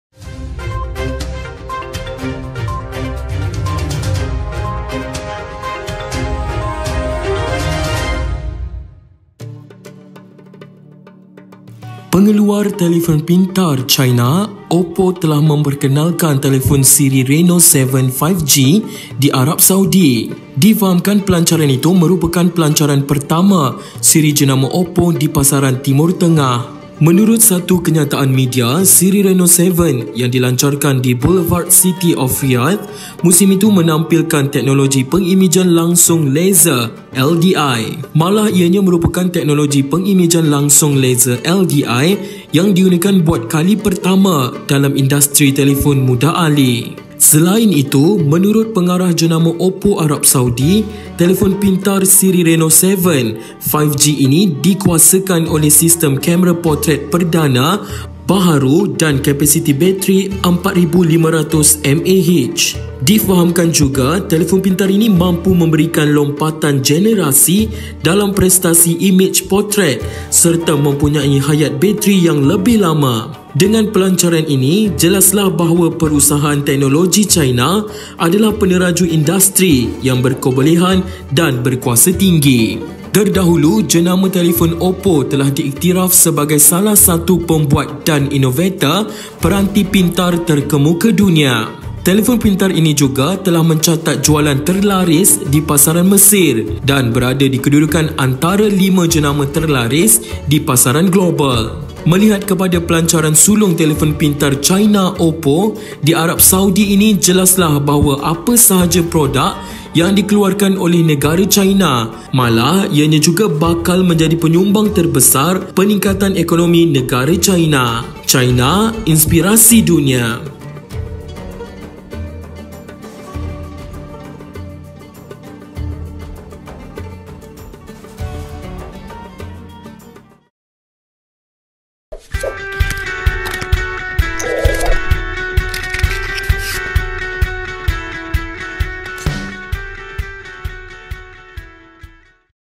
OPPO CHINA LANCARKAN TELEFON 5G BAHARU DI ARAB SAUDI !
Difahamkan pelancaran itu merupakan pelancaran pertama siri
jenama OPPO di pasaran Timur Tengah.